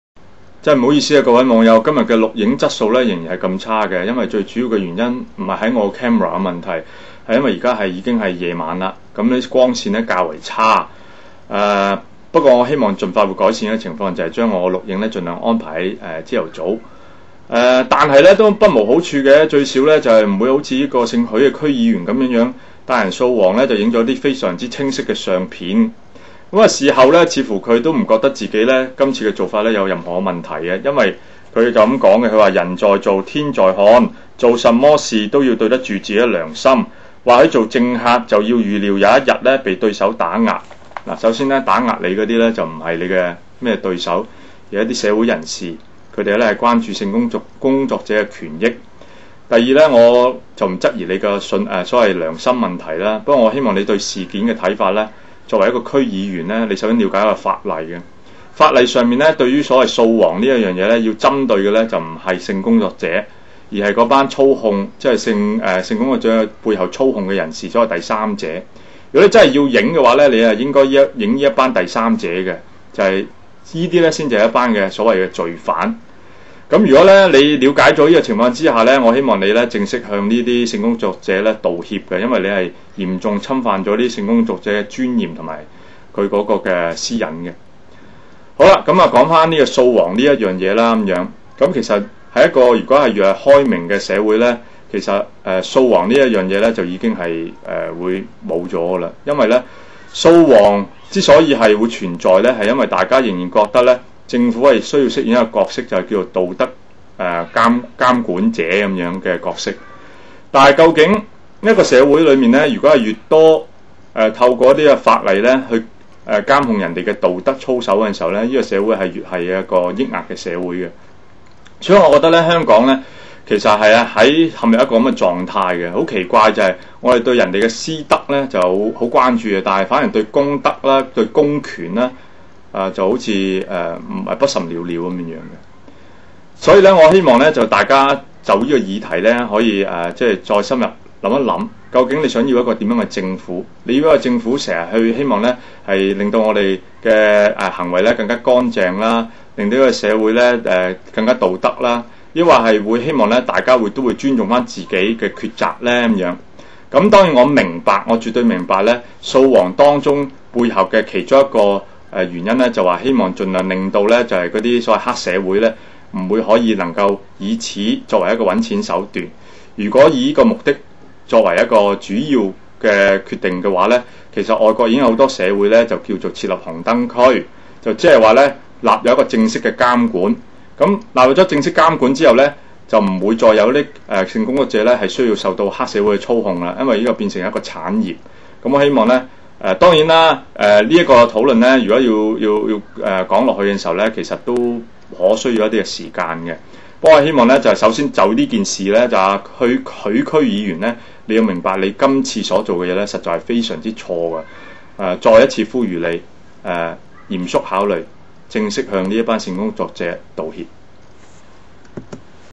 呼籲許德亮區議員盡快向性工作者道歉
油尖旺區議員許德亮帶隊掃黃，讓四名坐在「架步」床上的被捕女子任由傳媒拍照，損害這班女子的尊嚴，侵害他們的私隱。這名掃黃大隊長似乎是對掃黃的有關法例和背後的邏輯不甚了解，掃黃要打擊的操控妓女維生的人，其假設是妓女是受害者，她們需要的是被拯救出目前的困境，她們並非犯罪者，不是罪犯。事賓上，掃黄法例已不符合社會實情，一個開明、開放的社會將性工作合法化，設立紅燈區，這是保障性工作者不再受惡勢力操控和剝削的最佳辦法。